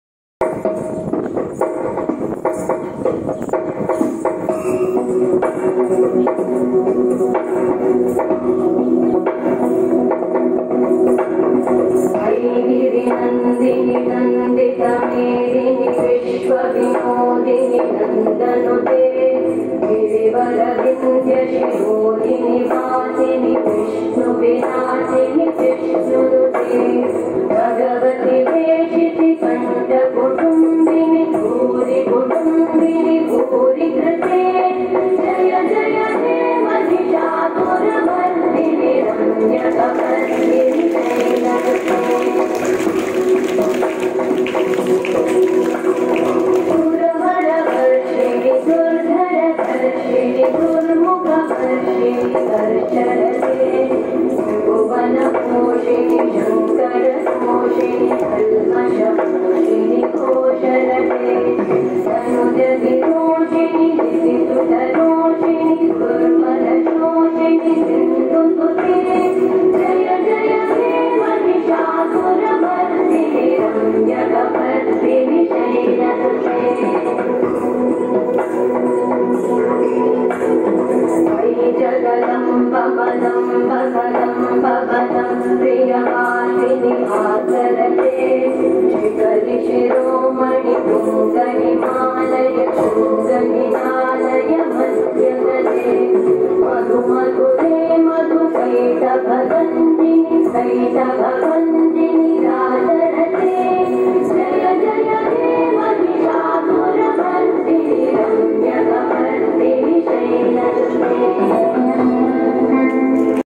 The eighth day of Navratri Durgaashtmi …2022
The eighth day of Navratri or the third day of Durga Puja, Maha Ashtami, celebrates Maa Durga's victory over the buffalo demon Mahishasura. People worship Maa Mahagauri on this day , who is known for bestowing her devotees with wealth and an opulent lifestyle, and removing all their sufferings. 🙏
Special Assembly